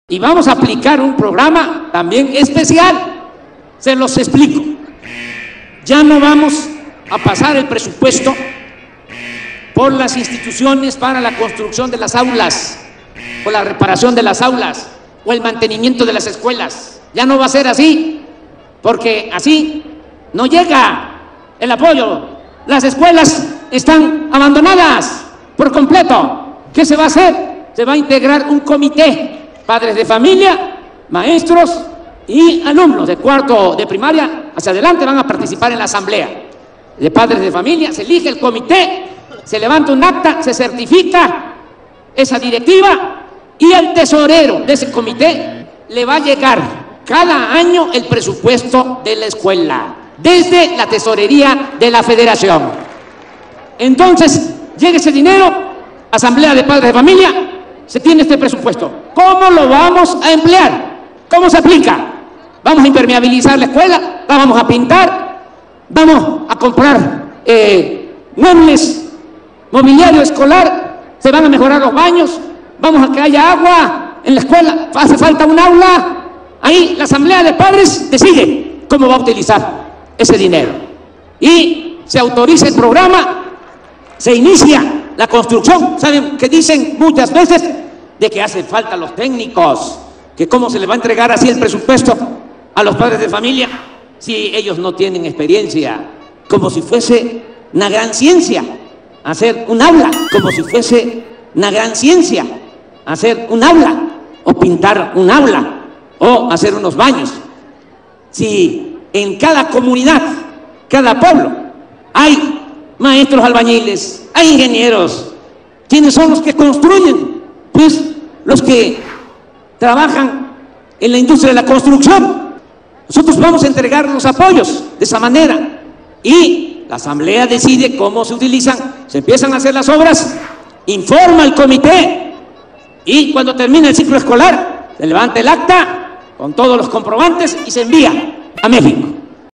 AMLO el destructor de Instituciones
‪¿Cómo alguien que quiere acabar con la corrupción de arriba y transformar a México se empeña en destruir instituciones e inconscientemente fomentar la corrupción abajo? arriesgando con eso la seguridad de los niños con tal de quedar bien dando apoyos directos! INIFED ‬